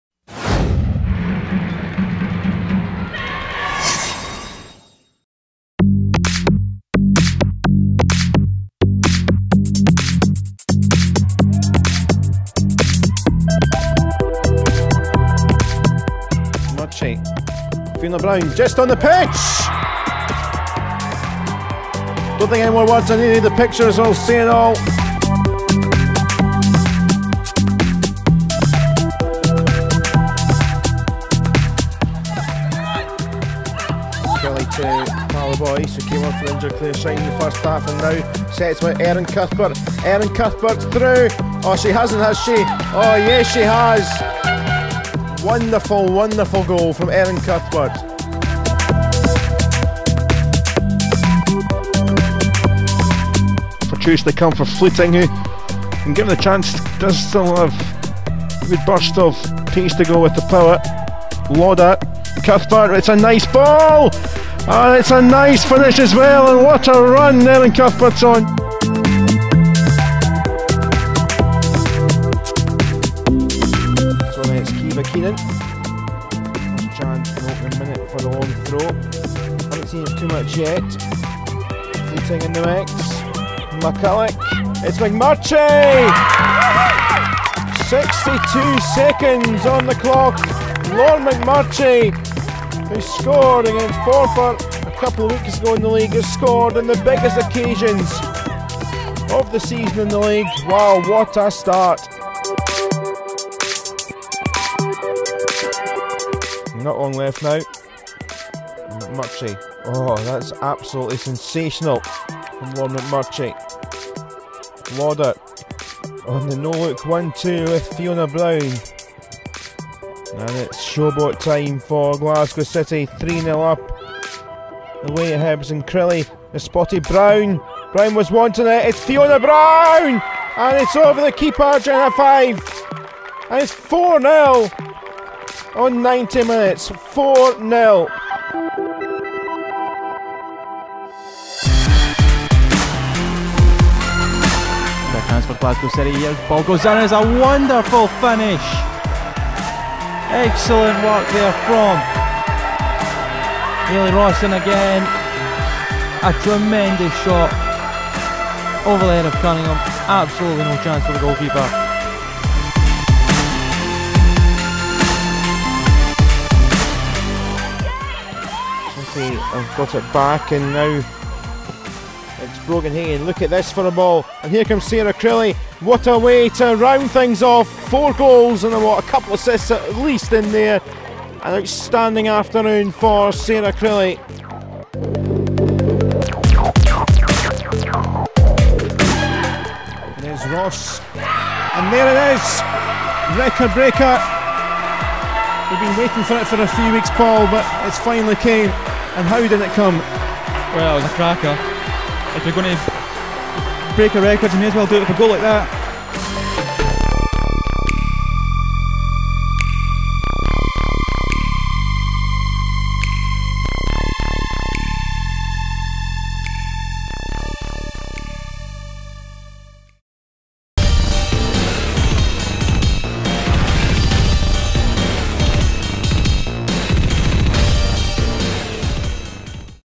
VOTING CLOSED | GOAL OF THE SEASON 2016
***VOTING CLOSED***

You can enjoy the 8 contenders for the 2016 GOTS.